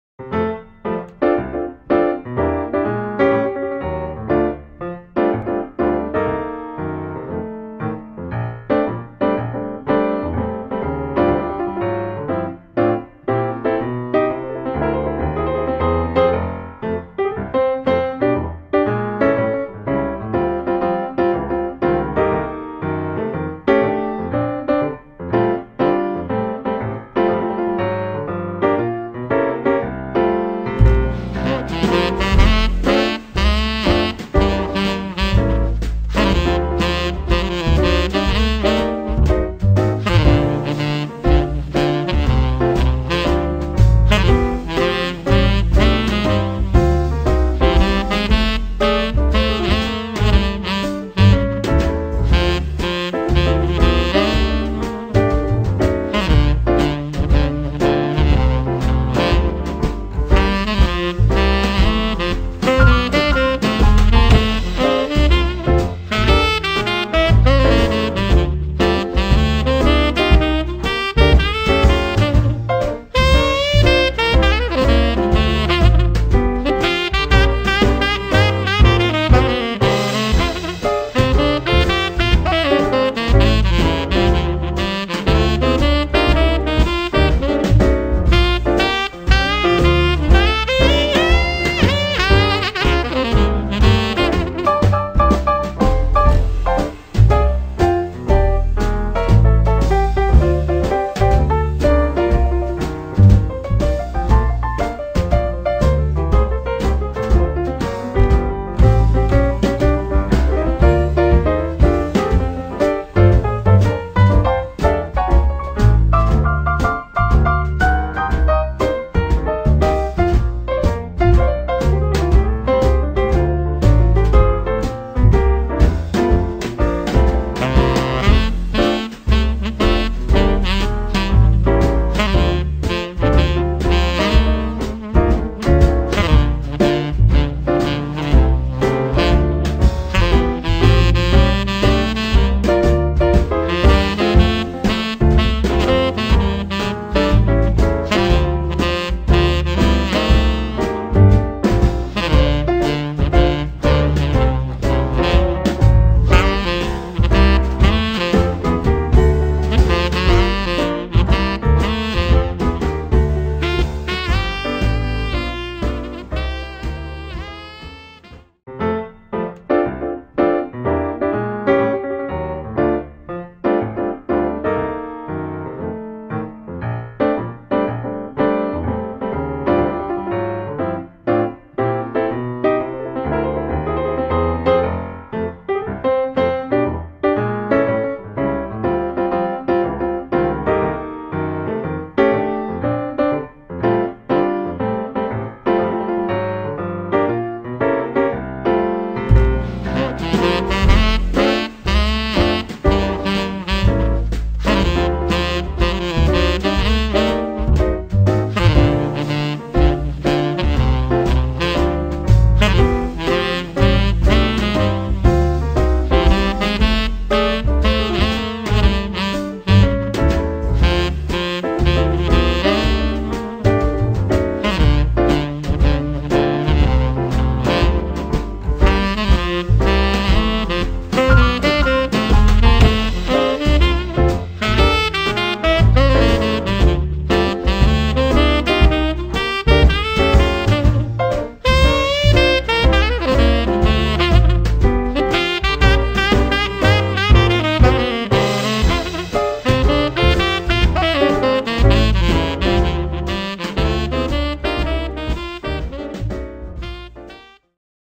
Модульное оригами. Котики. Мастер-класс.
Мастер-класс как сделать котиков из бумаги при помощи модульного оригами.